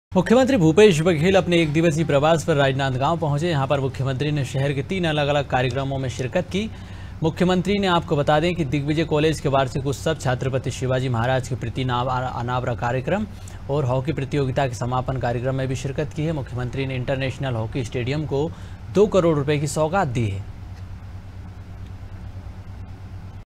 Rajnandgaon : सीएम बघेल का राजनांदगांव दौरा, कई कार्यक्रमों में की शिरकत || Anaadi Tv
मुख्यमंत्री भूपेश बघेल अपने एक दिवसीय प्रवास पर राजनांदगांव पहुंचे, जहां मुख्यमंत्री ने शहर के तीन अलग-अलग कार्यक्रमों में शिरकत की है.  मुख्यमंत्री दिग्विजय कॉलेज के वार्षिक उत्सव,छत्रपति शिवाजी महाराज के प्रतिमा अनावरण और हाँकी प्रतियोगिता के समापन कार्यक्रम में शामिल हुए मुख्यमंत्री ने इंटरनेशनल हॉकी स्टेडियम को 2 करोड रुपए की सौगात दी है, वही अंतरराष्ट्रीय हॉकी स्टेडियम में चल रहे फाइनल मैच में पहुंचकर खिलाड़ियों का उत्साहवर्धन किया, साथ ही सरकार की योजनाओं की जानकारी दी है..